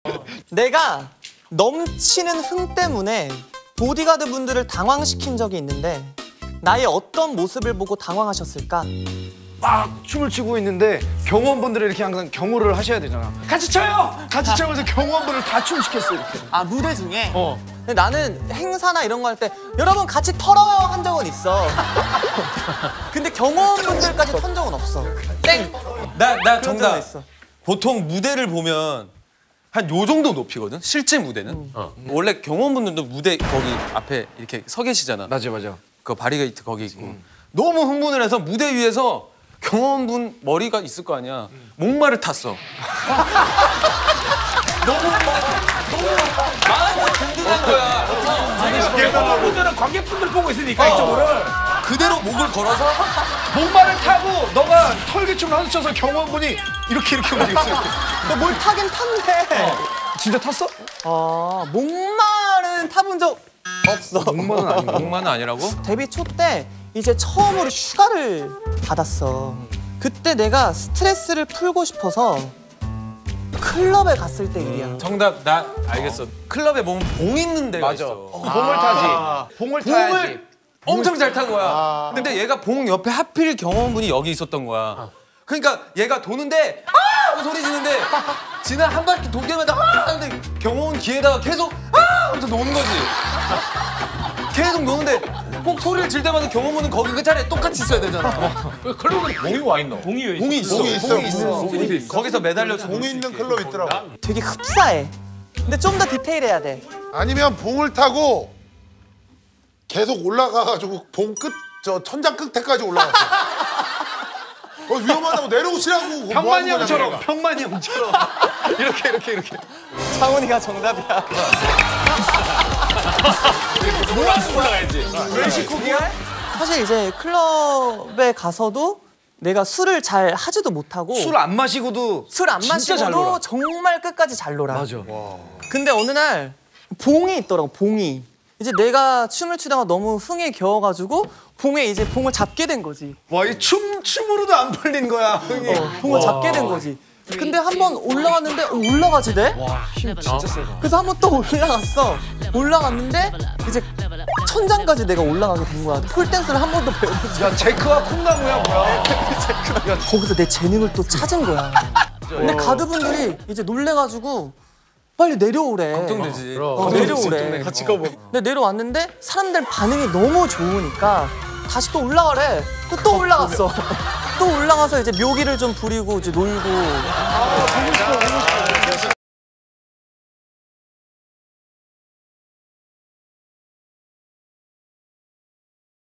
2am Jo-Kwon got way too excited on stage, he went on bodyguard's shoulder..?!
2am Jo-Kwon, "I guess i did clim something but i've never on someone's shoulders!"🤣